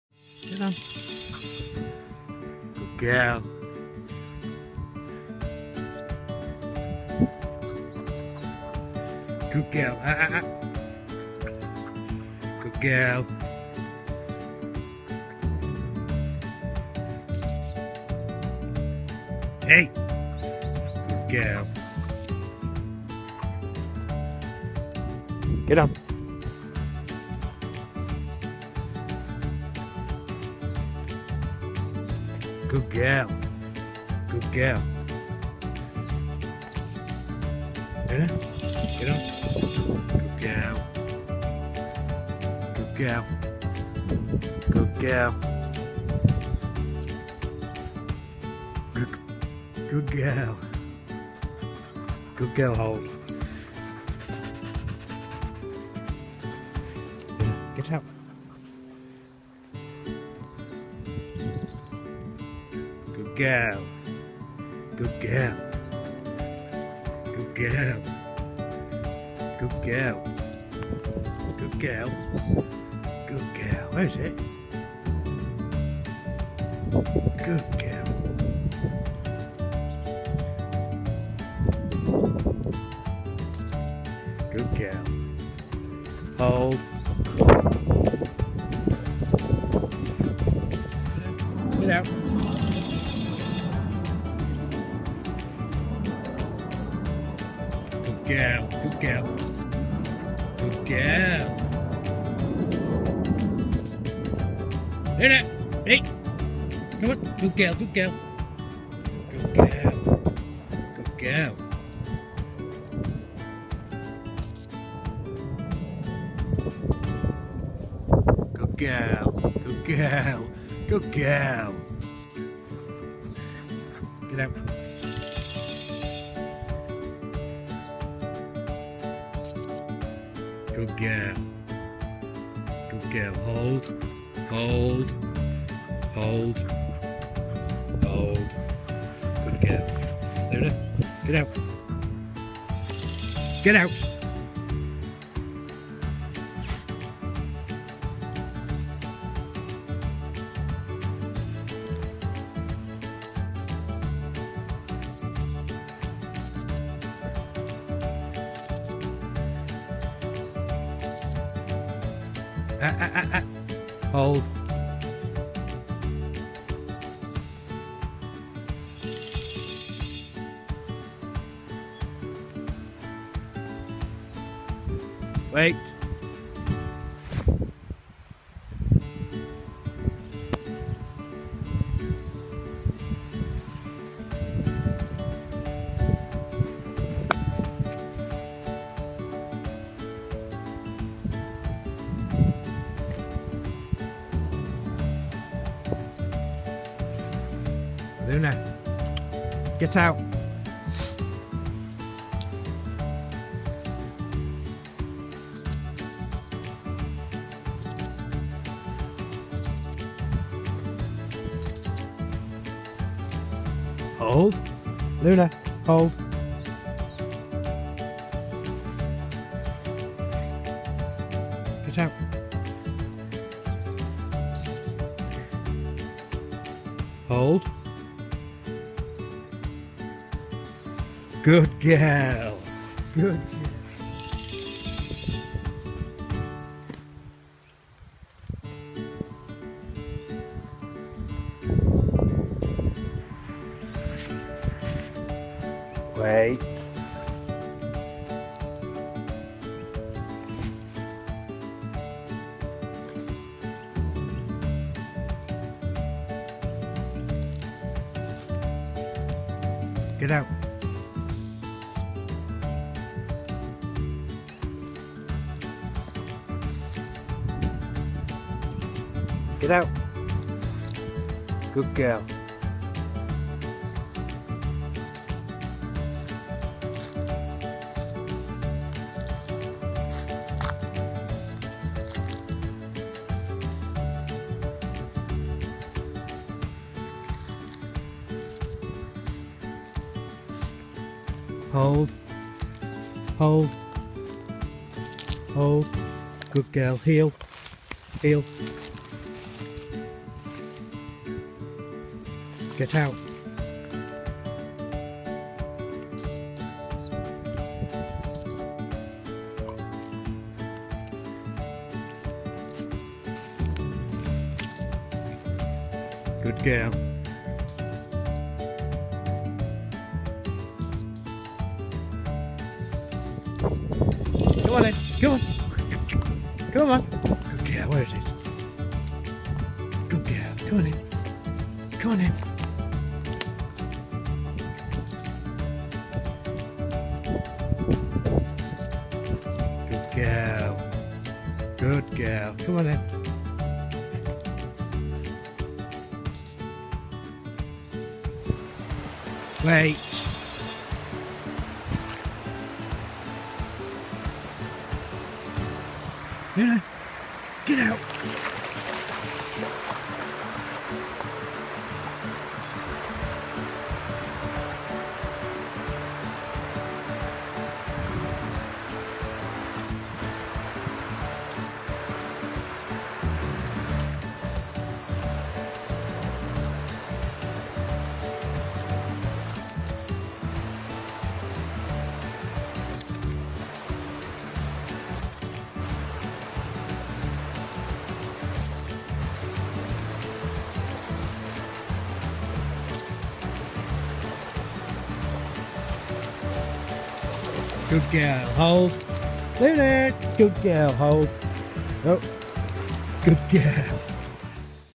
English Springer Spaniel Gundog training
This is Luna my 12 month old springer doing some gundog training